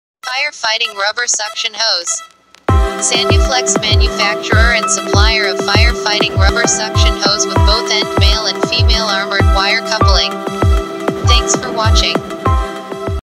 Fire Fighting Rubber Suction Hose
Fire are one of the leading manufacturers and exporters of 100 mm x 2.5 Mtr Fire Fighting Rubber Suction Hose. Sandhya Enterprises Manufacturer and Supplier of Fire Fights Rubber Suction Hose with Both end Male and Female armored Wire Coupling.
size: 100 mm ID x 2.5 Mtr Both end Male and Female Coupling as per IS 902 Specification.Working Pressure 10.0 kg/cm 2

Rs 11,500 / Number

Brochure

Minimum Order Quantity: 2 Number

Diameter: 100mm and 125mm

Single Hose Length: 2.5 m

Working Pressure: 10 kg/cm2

Warranty: 6 Month

Working Temperature: -10 to 150 Degree C

Brand: Sandhyaflex

Material: Rubber

We are a manufacturer and supplier of Fire Fighting Rubber Hose with Coupling.